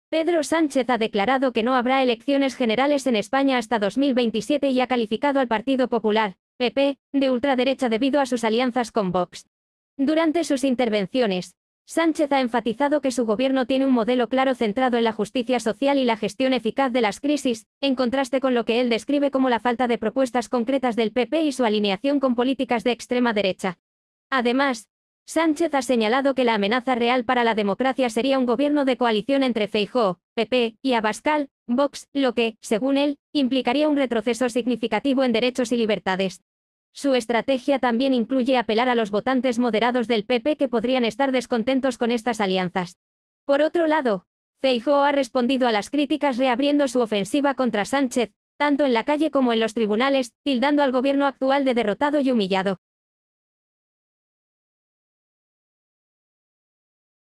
"Sánchez: No Habrá Elecciones Hasta 2027 y Califica al PP de Ultraderecha"
En este video analizamos las recientes declaraciones de Pedro Sánchez, donde afirma que no habrá elecciones generales en España hasta 2027 y acusa al Partido Popular de ser ultraderecha debido a sus alianzas con Vox. Sánchez destaca los logros de su gobierno y critica duramente la falta de propuestas del PP. También exploramos la respuesta de Feijóo, quien tacha al gobierno actual de "derrotado y humillado". ¡No te pierdas este análisis completo de la situación política actual en España.